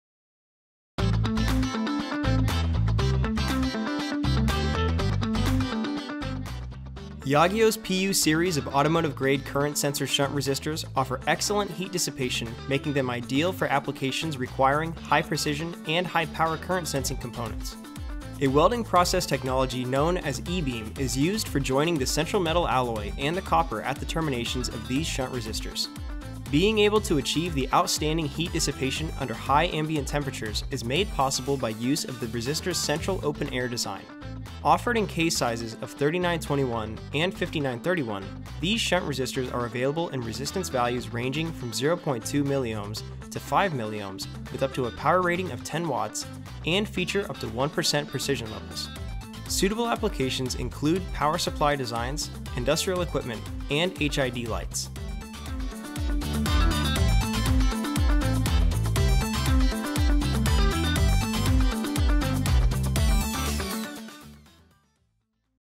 Yageo PU Series Shunt Resistors | Digi-Key Daily
Yageo's PU series of automotive-grade current sensor shunt resistors offer excellent heat dissipation, making them ideal for applications requiring high-precision and high-power current-sensing components. A welding process technology, known as E-beam, is used for joining the central metal alloy and the copper at the terminations of these shunt resistors. Being able to achieve the outstanding heat dissipation under high ambient temperatures is made possible by use of the resistor's central open-air design. Offered in case sizes of 3921 and 5931, these shunt resistors are available in resistance values ranging from 0.2 milliohms to 5 milliohms with up to a power rating of 10 watts, and feature up to 1% precision levels. Suitable applications include power supply designs, industrial equipment, and HID (pronounced: "H-I-D") lights.